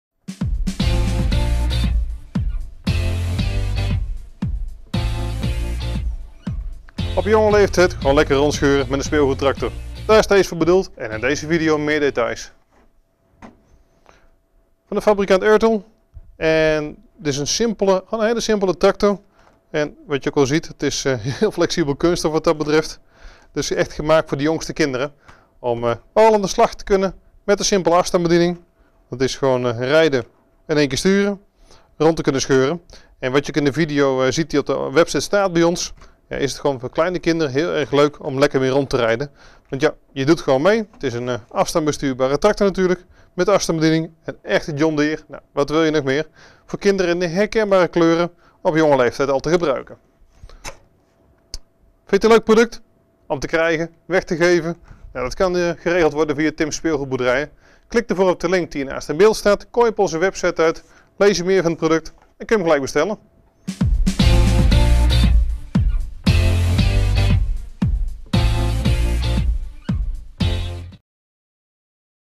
REMOTE CONTROL TRACTOR JOHNNY Artikelnr: 42946
Deze afstandsbestuurbare tractor van Britains is speciaal gemaakt voor kinderen vanaf 1,5 jaar.

Door de simpele bediening van de tractor met 2 knoppen kunnen de allerkleinsten deze tractor gemakkelijk vooruit en achteruit laten rijden.

De tractor is vormgegeven in de John Deere kleuren in de typische Britains Tomy stijl.